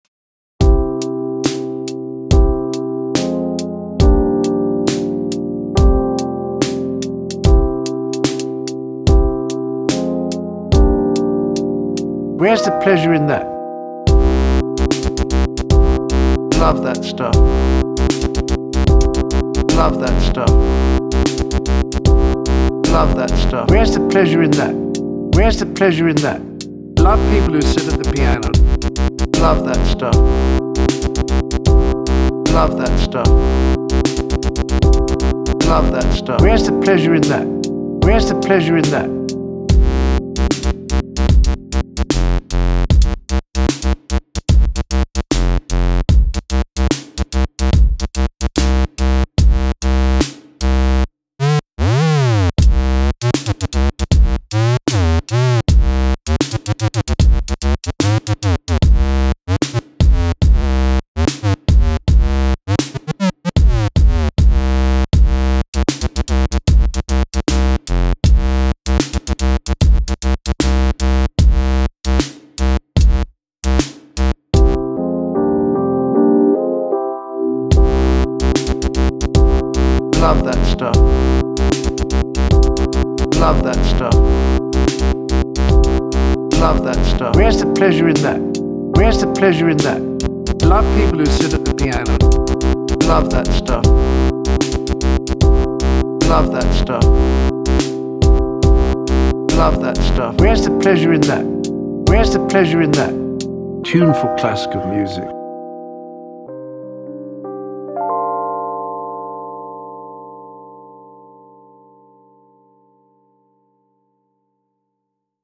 MIDI Fighter 3D, Reason & John Cleese
Sampled a bit of good 'ol John Cleese, built a nice Kong set around it in Reason and banged a tune out live with the MIDI Fighter 3D.